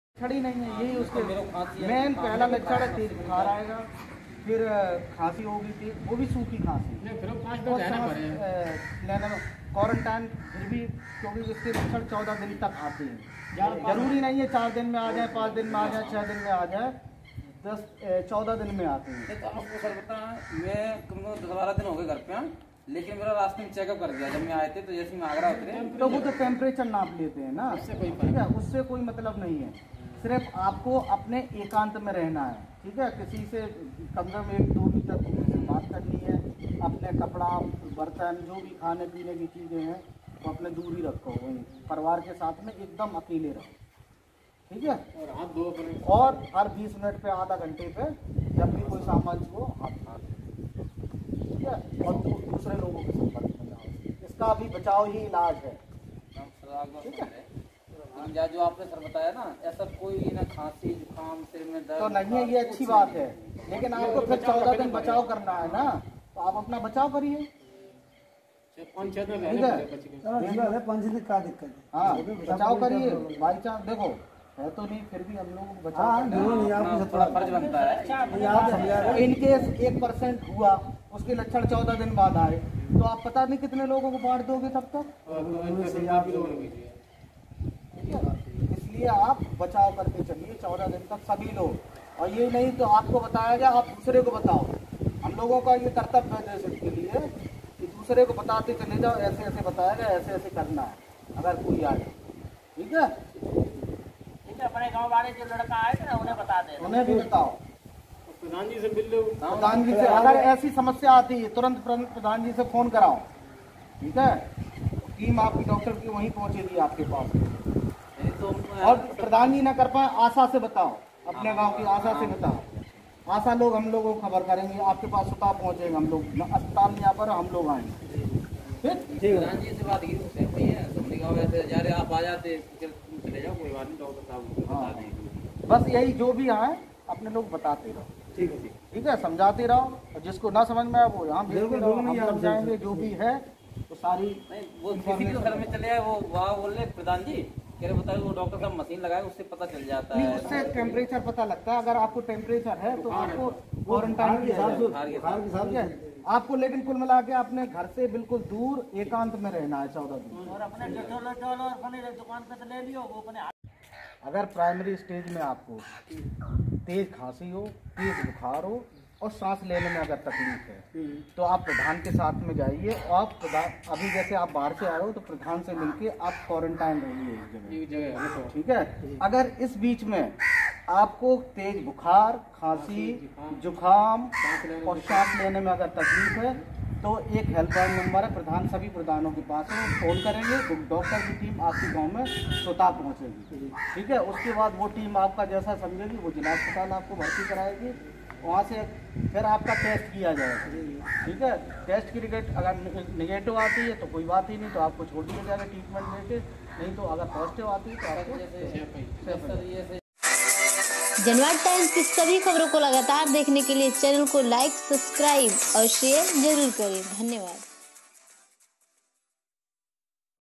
Coronavirus Awareness : डॉक्टर नीरज दुबे सामुदायिक स्वास्थ्य केंद्र वैदपुरा सैफई इटावा
Janvad Times : Coronavirus Awareness :  डॉक्टर नीरज दुबे सामुदायिक स्वास्थ्य केंद्र वैदपुरा सैफई इटावा ने दिया कोरोना वायरस के संबंध में जानकारी ।

Coronavirus Awareness live updates, current update coronavirus awareness, current update coronavirus awarness,
Latest update Corona awareness,

सामुदायिक स्वास्थ्य केंद्र वैदपुरा सैफई इटावा के डॉक्टर नीरज  दुबे  प्रदेश के बाहर से आए हुए महेश चंद पुत्र अमृत प्रकाश और लक्ष्मण सिंह पुत्र अमृत प्रकाश निवासी वैदपुरा को कोरोना वायरस के संबंध में जानकारी देते हुए  उन्होंने कहा कि आपको 14 दिन तक अपने आपको क्वॉरेंटाइन रहना है ।आपको  बचाव अधिक करना है । बचाव करना ही अभी इसका इलाज है और आपको दिन में चार बार गरम पानी पीना है। डॉ राजेंद्र सिंह , डॉ भावना त्रिपाठी फार्मेसिस्ट विकास मौजूद रहे